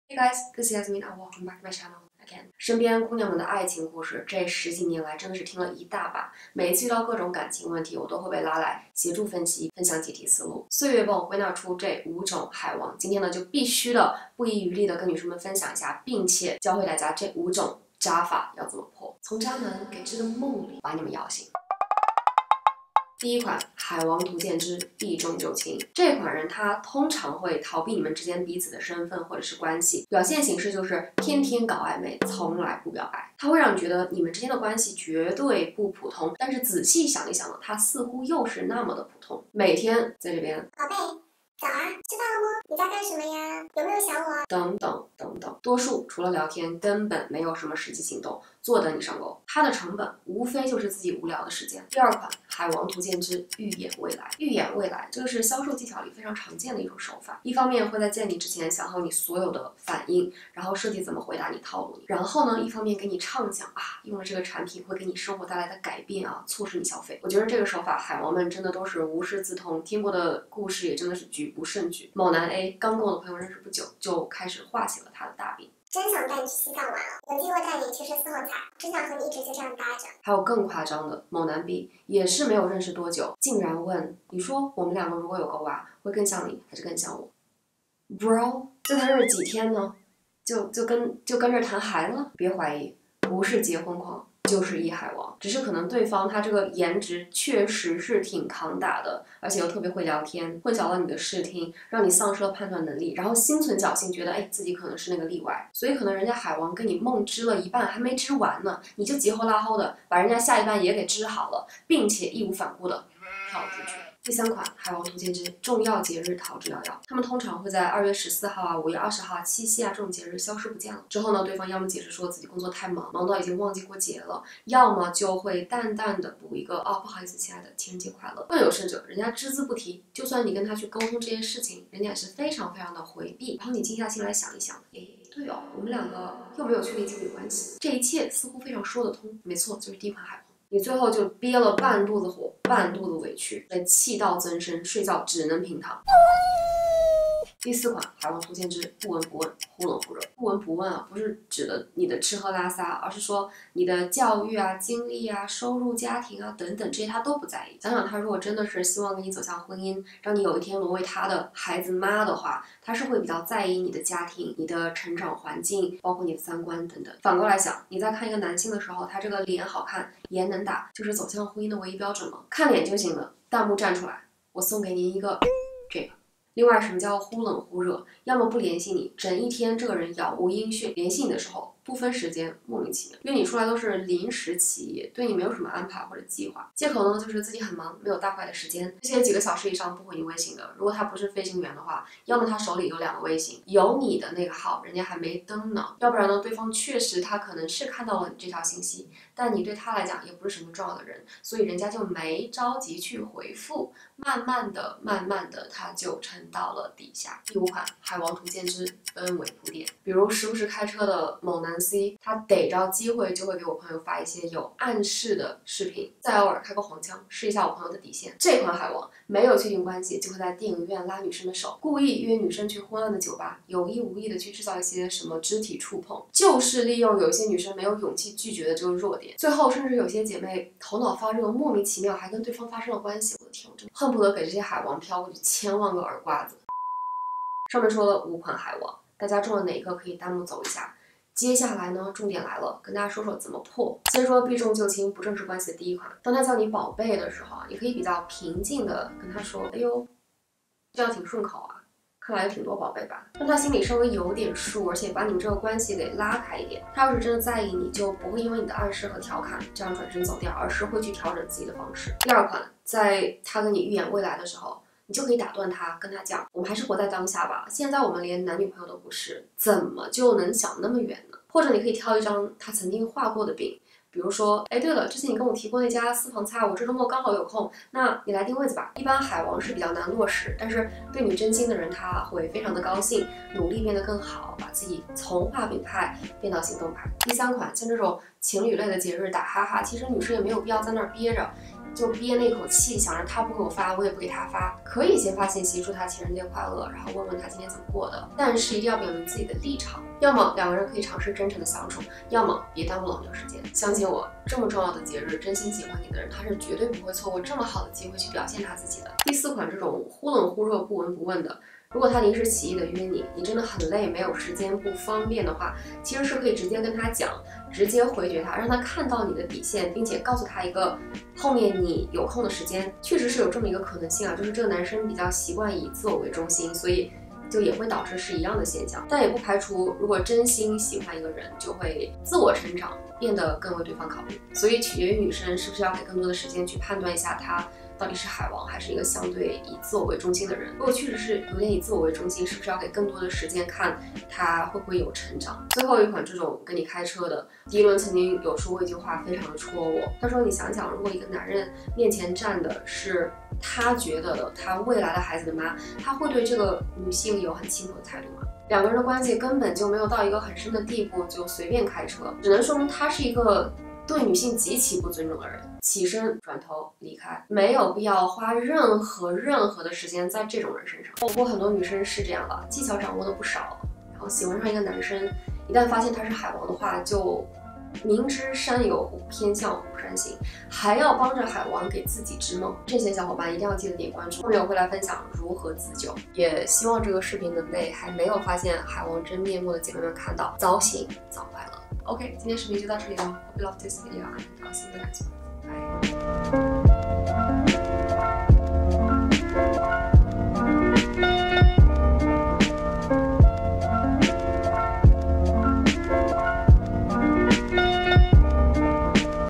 5种海王套路图鉴! 从渣男给织的梦里把你们摇醒，教你见招拆招
卡娘娘热线又开启啦～今天的恋爱小课堂教你识破这几款海王！见招拆招，早醒早快乐！